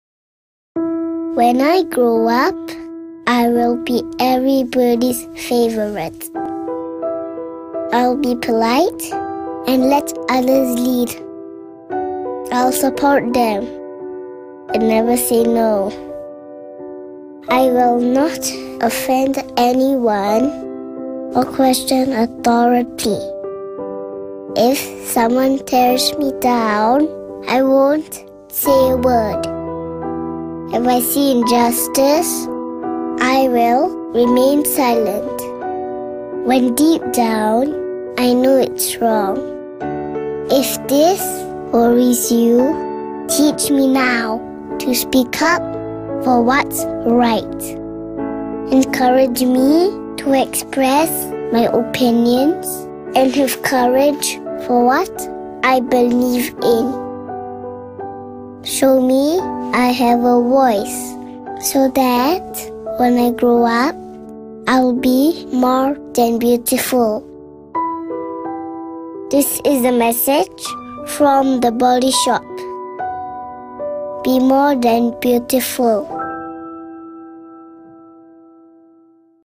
Khi Con Lớn Lên, Con Sẽ Biết Cất Tiếng Nói
Chiến dịch Be More Than Beautiful - Vẻ Đẹp Trọn Vẹn

Đoạn video chân thực và cảm động đến không ngờ với diễn đạt tự nhiên của một cô bé nhi đồng khiến chúng ta đong đầy suy nghĩ.

Là đoạn video cho chiến dịch "Be More Than Beautiful - Vẻ Đẹp Trọn Vẹn" của The Body Shop, chúng tôi muốn truyền tải đến mọi người thông điệp thực thụ về cái đẹp, để đẩy lùi những quan điểm sai lầm mà giới trẻ ngày nay lầm tưởng.

Chúng tôi, tại The Body Shop, không xem cái đẹp một cách bị động, cứng nhắc như quan niệm về ngoại hình. Với chúng tôi, vẻ đẹp là một phần tích cực trong cuộc sống hàng ngày. Nó được quyết định bởi tính cách, trí tưởng tượng và cả khiếu hài hước của bạn. Ngắn gọn hơn, bạn đẹp khi bạn tự hào mỉm cười với thế giới, bạn yêu bản thân mình đến đâu.